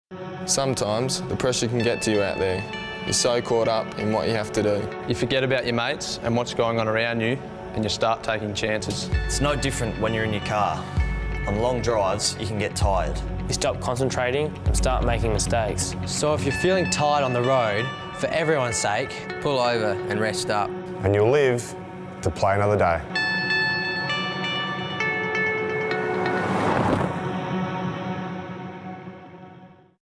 Avoid Driving Fatigued - Be a MAC Game Changer
When travelling, rest for 15 minutes every two hours to be a Game Changer on the road. Be a hero on the footy field, not on the road.